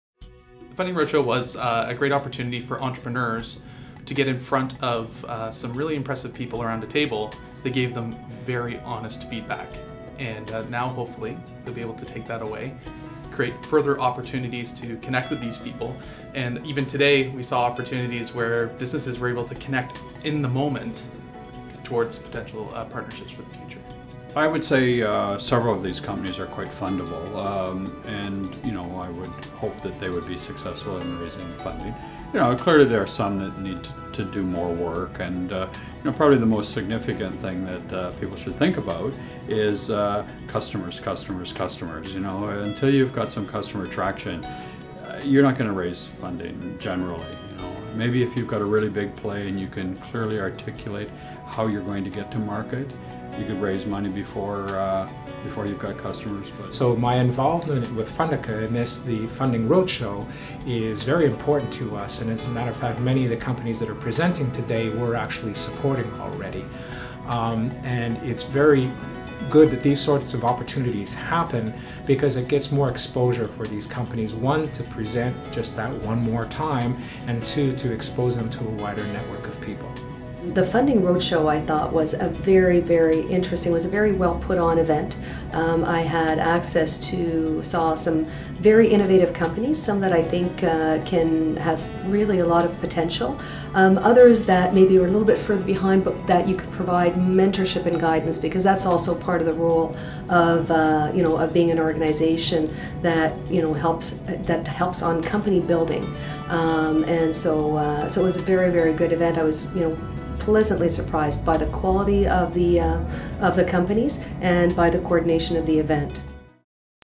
The 2013 Funding Roadshow - Ottawa Funders
The 2013 Funding Roadshow brought together 103 entrepreneurs and 35 funders from 5 Canadian cities. As a result, 39% of the entrepreneurs who pitched were offered funding. Not bad for a first year!